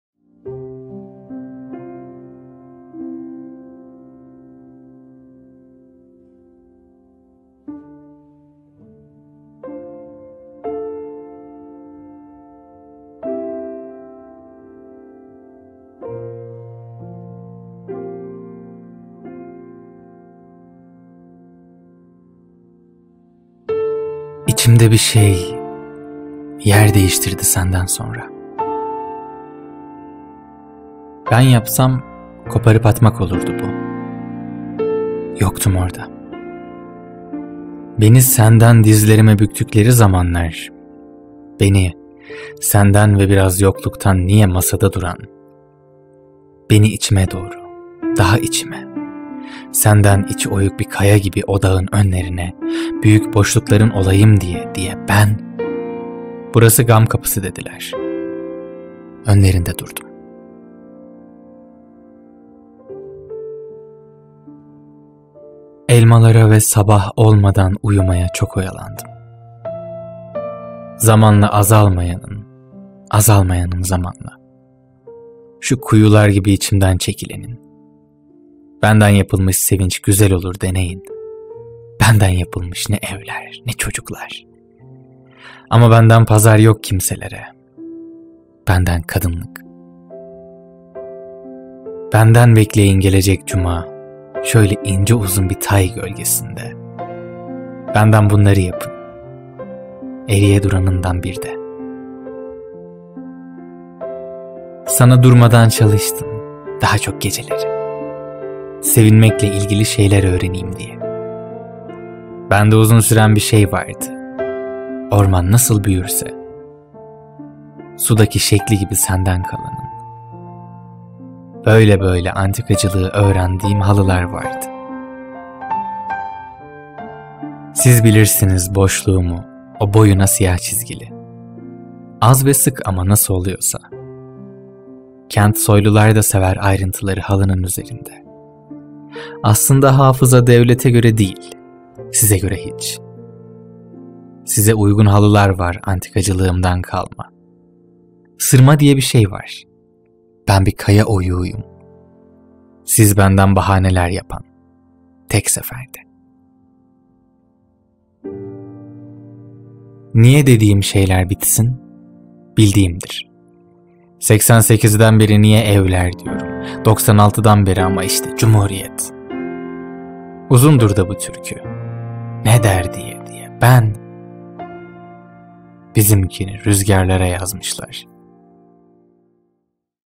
Evindar - Altay Kenger
Âşık Olduğum Kadınlar Antolojisi: Birinci Cilt

Merhaba, dizelerinde dönüp durduğum çok sevgili kadınları bir araya getirmeyi amaçladığım bu çalışma, aynı zamanda en geniş çalışmam oldu. Tek parça halinde yayımlamak bazı şairlerin gözden kaçmasına neden olur diye endişe edip, üç cilt halinde yayınlamaya karar verdim. Adlarını andığım, şiirlerini okuduğum bu güzelim şairlere elimden geldiğince ulaşıp icazet almaya çalıştım. Ulaşamadıklarım oldu, benden razı olsunlar, bu çalışma sadece çok sevdiğim için emek verdiğim bir çalışma.

Âşık olduğum kadınların dizelerini dinlerken, umuyorum ki, onları okumaya meyledenleriniz çok olur. Onları okumanızı, benden dinlemenizden daha çok isterim.

Neyse, sürç-ü lisan ettimse hoş görüle, hoş şiirler duyarken elbet..